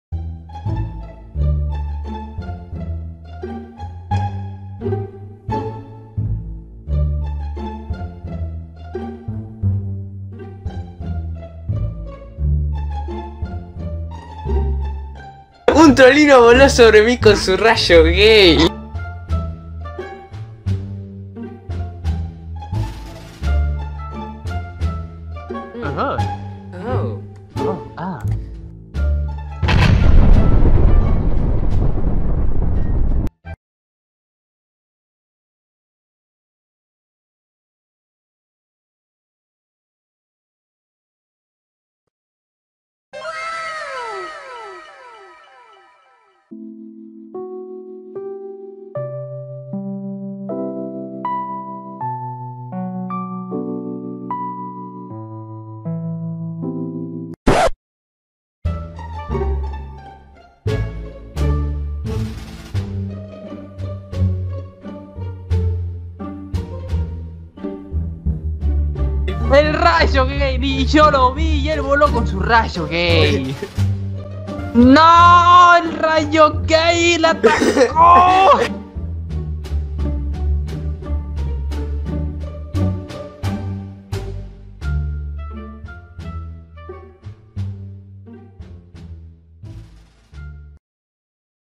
UN TROLLINO VOLÓ SOBRE MI CON SU RAYO GAY 😂 | ¿CREACIÓN DEL MIKELLINO? 😱
y así fue como surgió el mikellino, buenas noches.

creadora: maibel casi anime!!TwT

atte: casmy 👌